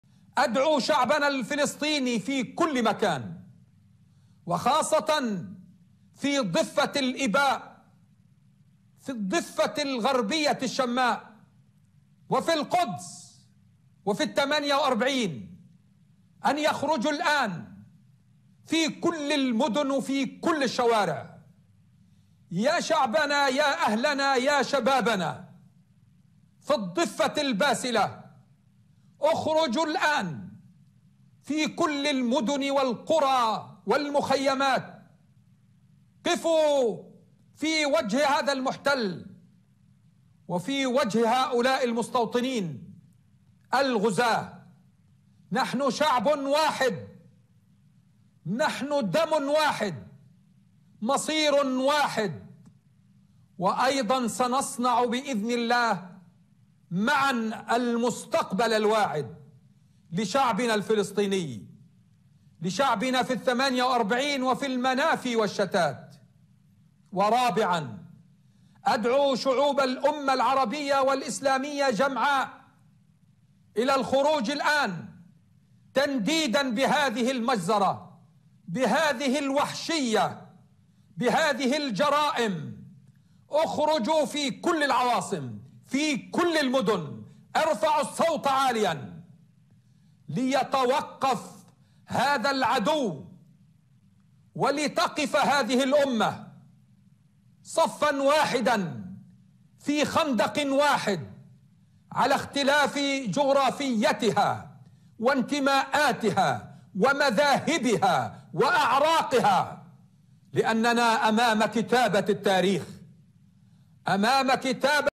إسماعيل هنية: أدعو شعبنا خاصة في الضفة للوقوف بوجه الاحتلال وأقول للشعوب العربية أخرجوا في كل مكان
- هنية:  ثقتنا بالسعودية وكل الدول العربية والإسلامية عالية بأن هذا الدم لن يضيع هدرا وأن الاجتماعات لن تمر مرور الكرام
- هنية: أدعو إلى إدانة دولية وعربية واضحة لمجزرة ولا يجوز للصمت أن يستمر
- هنية: إسرائيل واهمة إذا ظنت أن المجازر والمذابح ستدفع الفلسطينيين للاستسلام
-هنية: المقاومة مستمرة ولن تتوقف إلا برحيل المحتلين عن أرضنا ومقدساتنا